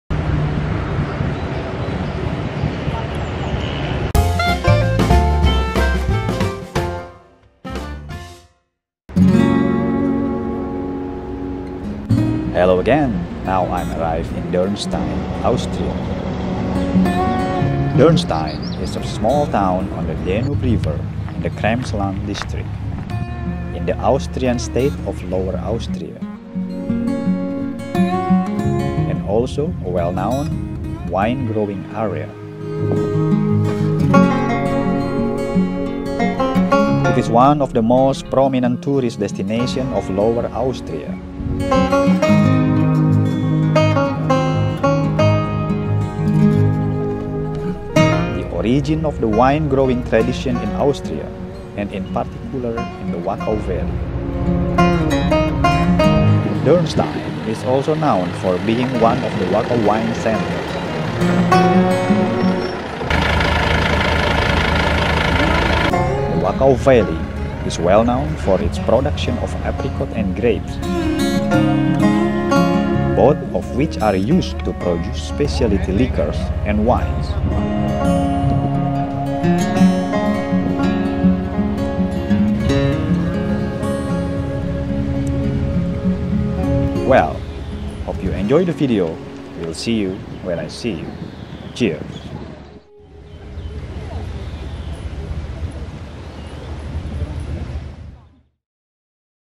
Durnstein Austria in 2 Minutes
Durnstein Austria in 2 minutes will show you the small town by danube river.

Dürnstein is a small town on the Danube river in the Krems-Land district.
In the Austrian state of Lower Austria.

And also a well-known wine growing area.

It is one of the most prominent tourist destinations of Lower Austria.

The origin of the wine growing tradition in Austria, and in particular in the Wachau valley.

Dürnstein is also known for being one of the Wachau wine centres.

The Wachau valley is well known for its production of apricots and grapes.

Both of which are used to produce specialty liquors and wines.

Well hope you enjoy the video, will see you when i see you.

Cheeerrss.